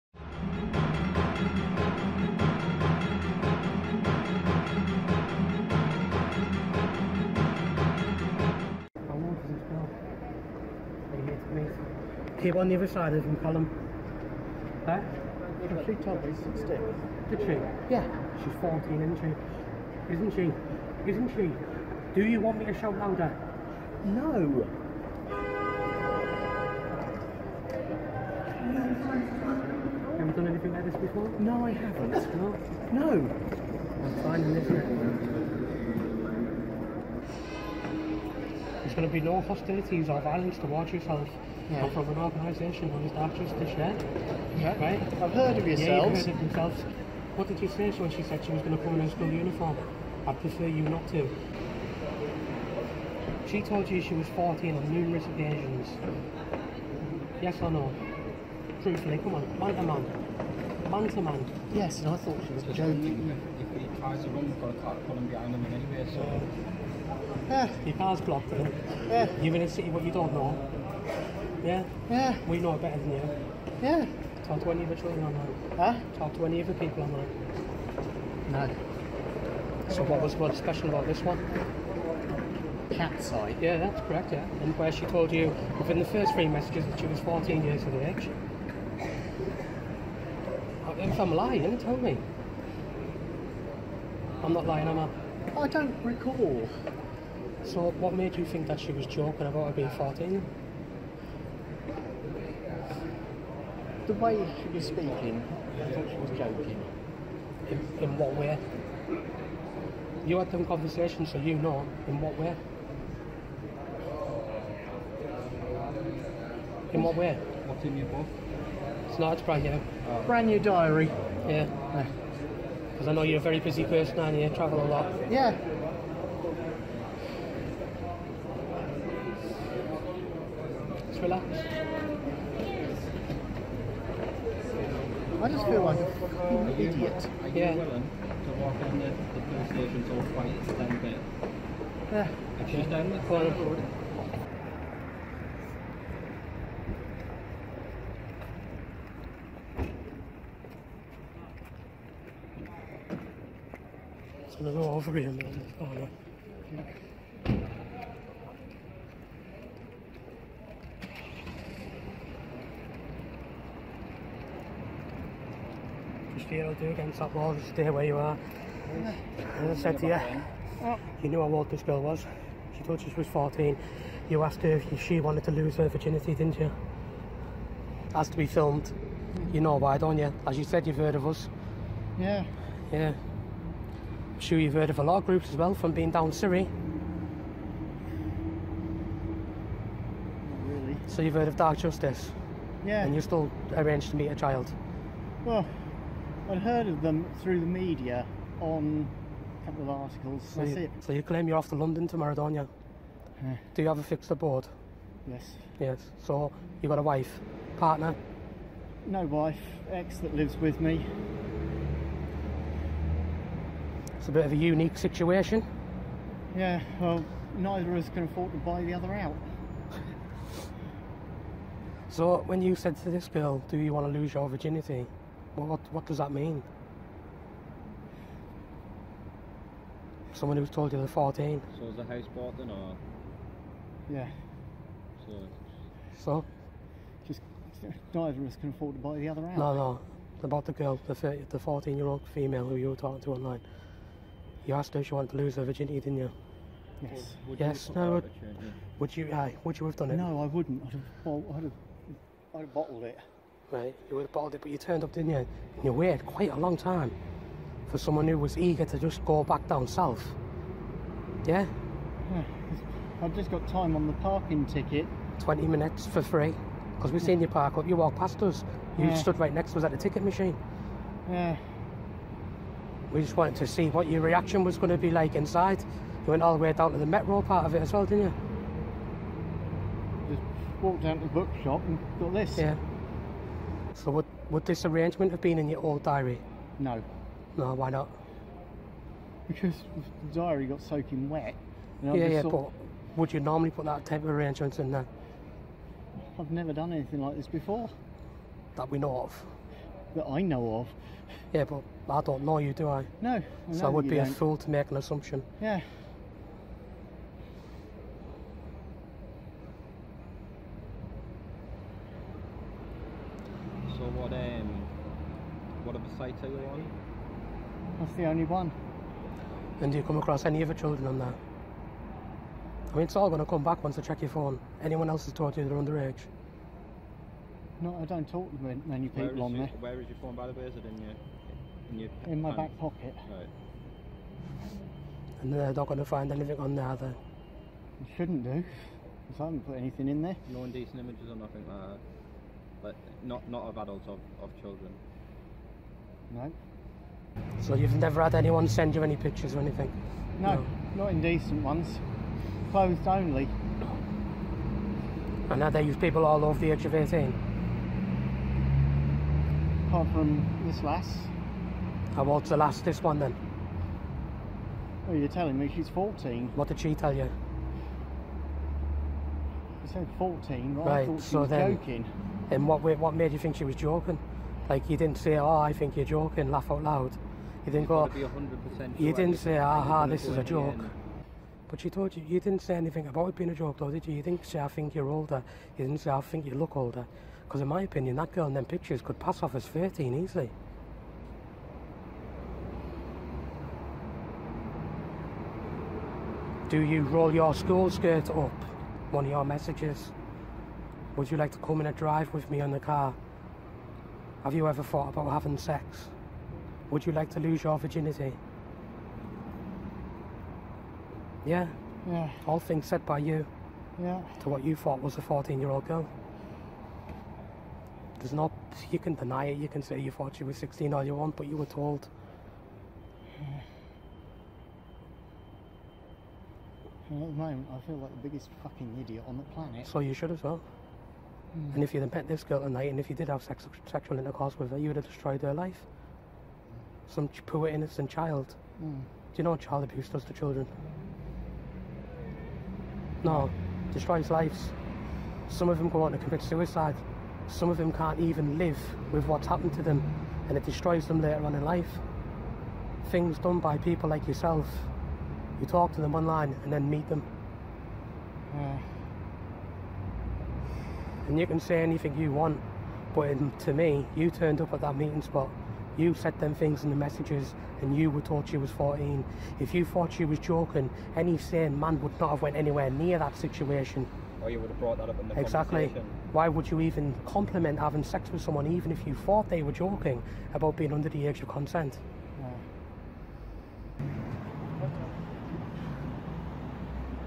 Dark Justice: Tony Watson caught trying to meet 14 year old girl
THIS VIDEO DOES NOT BREACH ANY OF YOUTUBE'S RULES AND YOU CAN SEE THE OFFICIAL PRESS RELEASE FOR THIS CASE

This video is of when Tony Watson was caught trying to meet a child following sexual grooming! he has now been convicted in a court of law and you can view the official mainstream media of this story here:

And this video does NOT breach any of YouTube's rules nor any UK law!